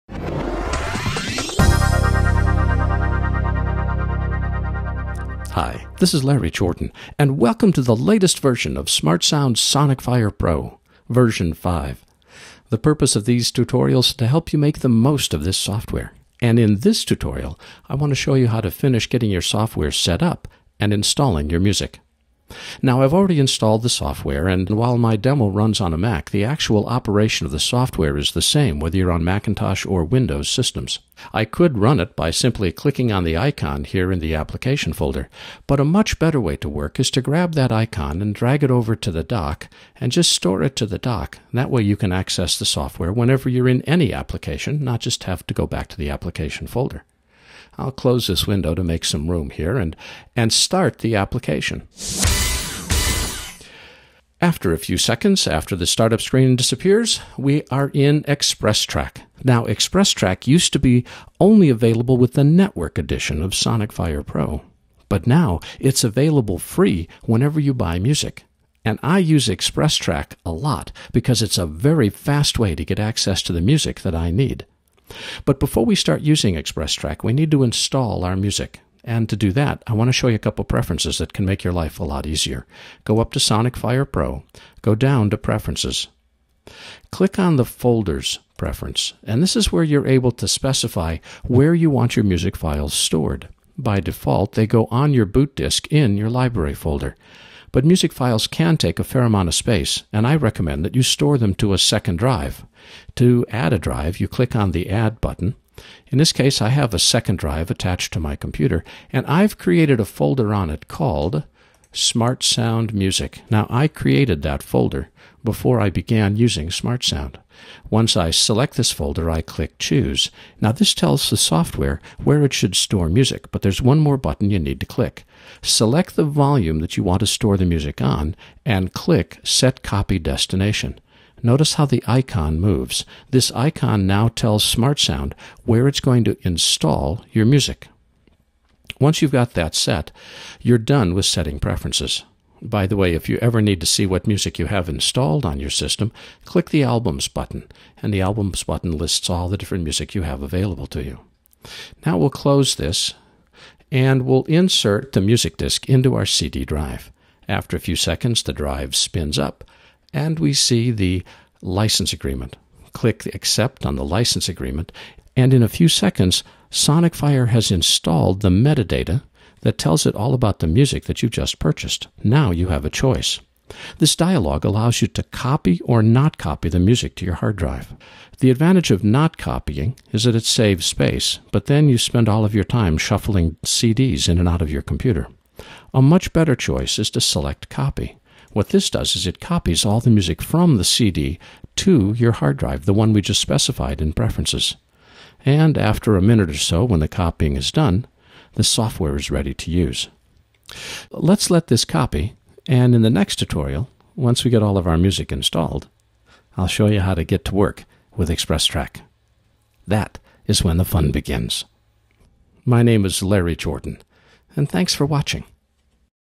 1. Getting Started with Sonicfire Pro 5 Express Track Edition
In this tutorial, Final Cut guru, Larry Jordan - shows you how to get your Sonicfire Pro 5 software installed and setup.

Learn how to use SmartSound Sonicfire Pro 5 to turn library music into a custom music score for your films and videos. Filmmakers, video producers, video editors, music editors, animators and other creative content producers now have an affordable method for adding high-impact music to their productions.